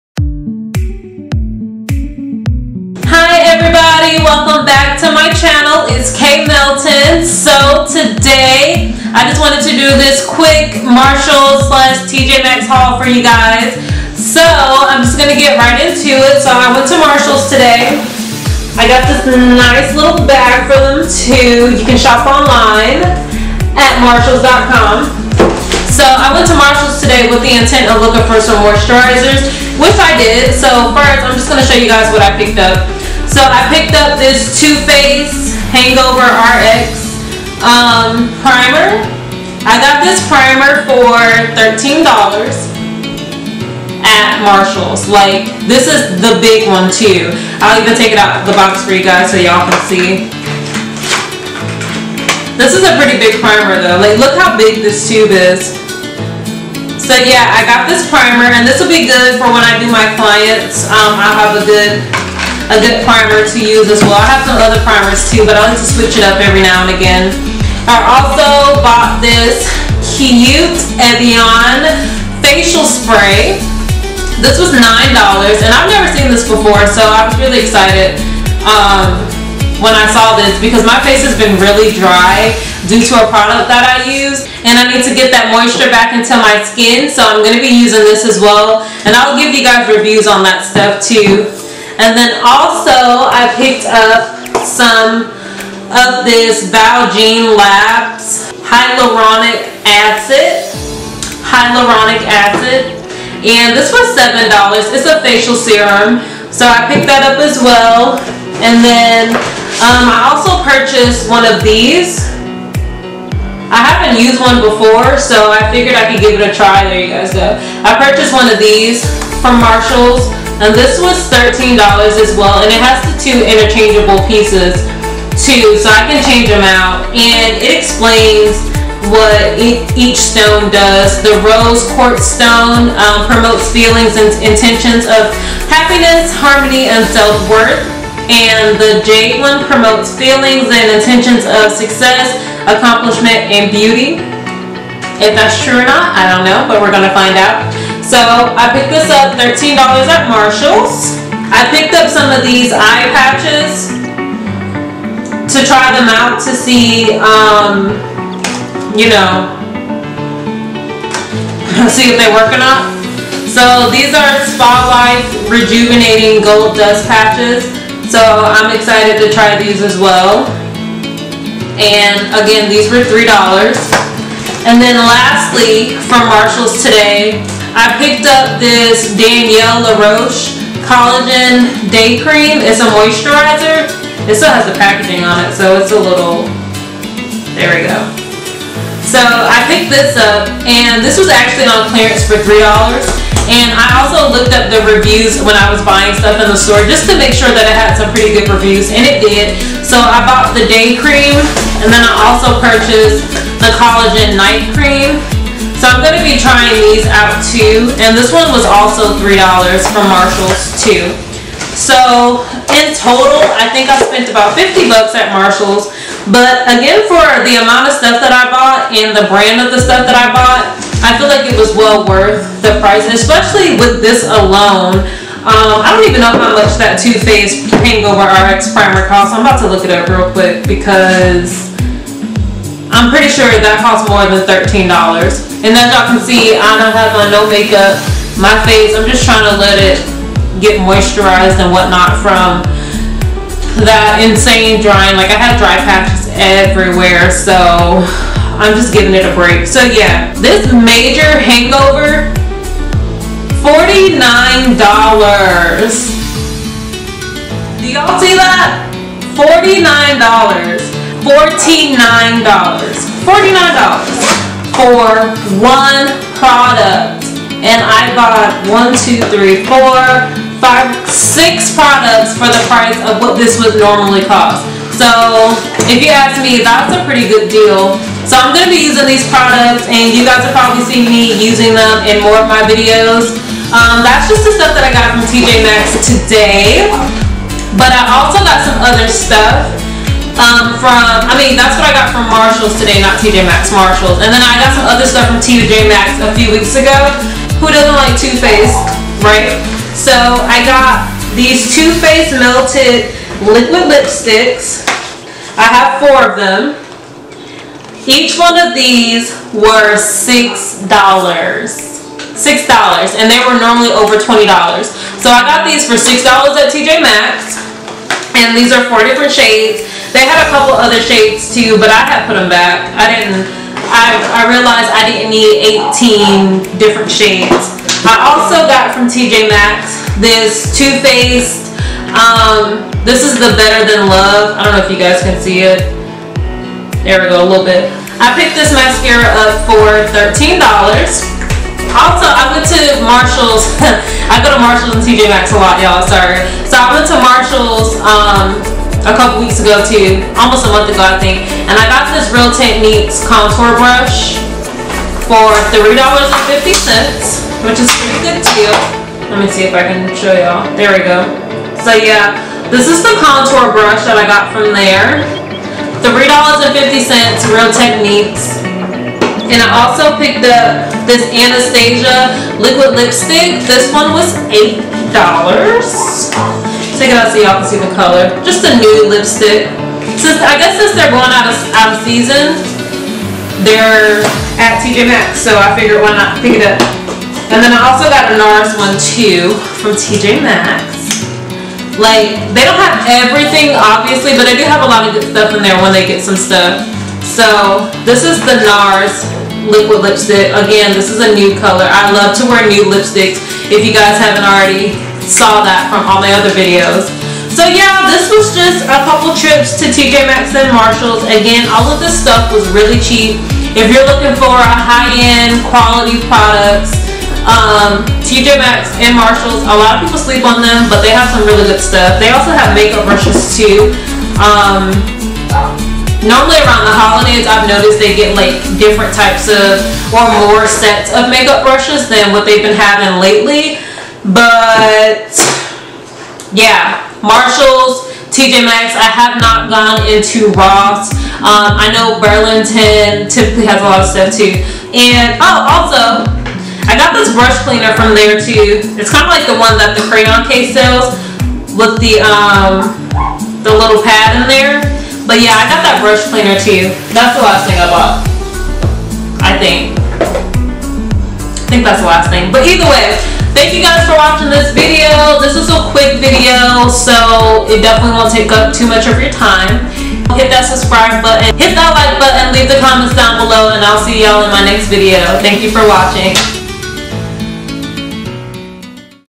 Marshalls and TJ Maxx Haul | K. Melton
Hey Y'all! I hope you enjoy this Marshalls and TJMaxx haul video. I got some pretty Good items for a great price. The product links will be listed below with the original prices so you all can see the difference in the pricing!

-Evian Facial Spray
-Val Jean Labs Hyaluronic Acid Facial Serum
-Daneille Laroche Collagen Day Cream and Night Cream
-Too Faced Better Than Love Mascara